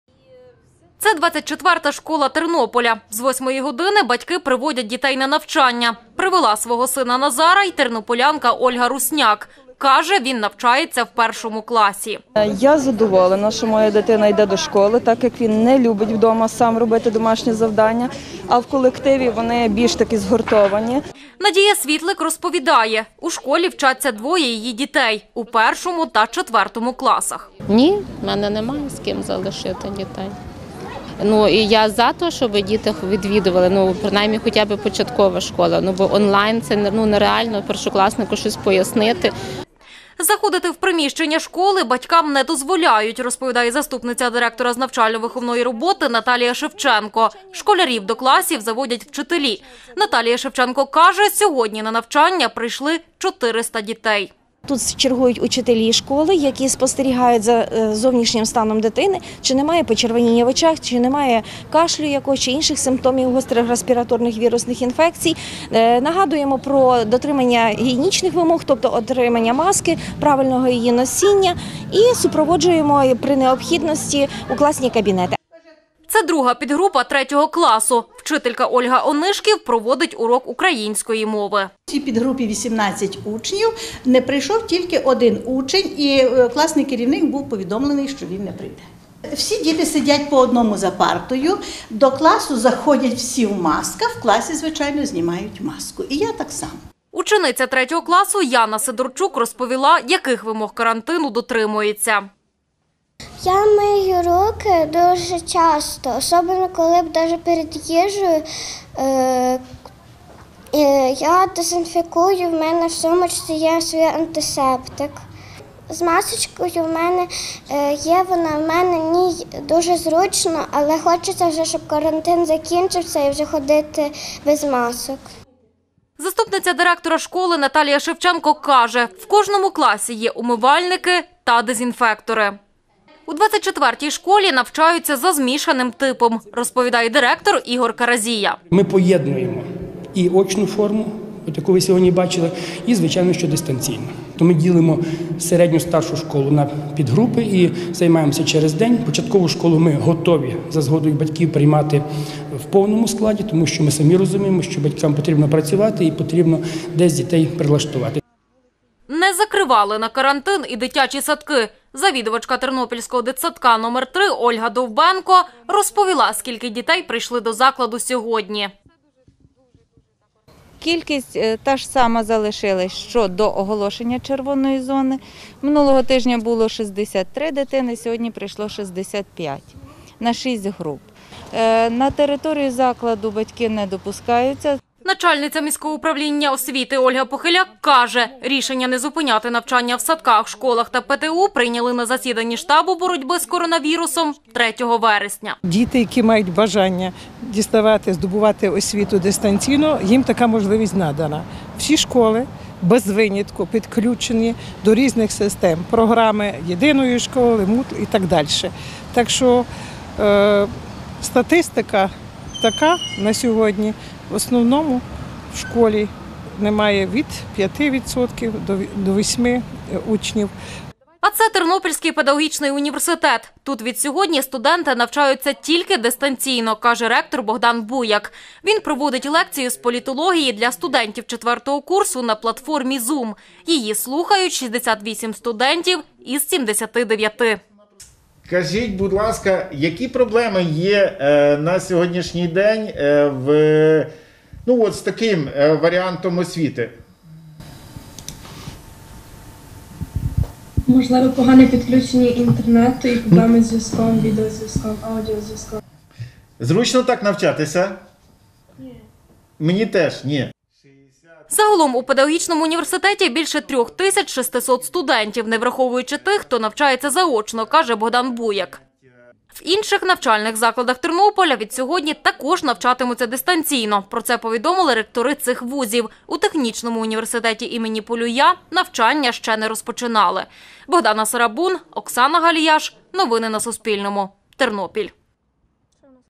У чотирьох вчителів тернопільської школи №6 підтвердили коронавірус
У чотирьох вчителів тернопільської школи №6 підтвердили COVID-19. Від сьогодні, 7 вересня, в школі тижневі канікули. Про це повідомив директор навчального закладу Олександр Остапчук